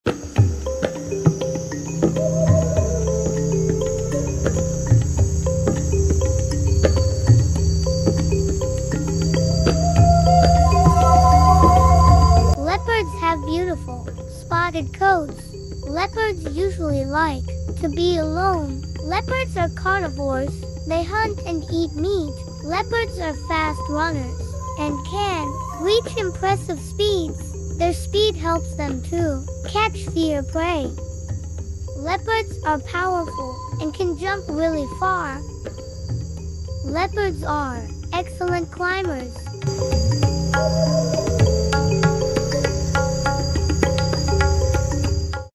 Did you know........
some fun facts about leopard. Tell your kids about beauty of animals with Kids Adventure.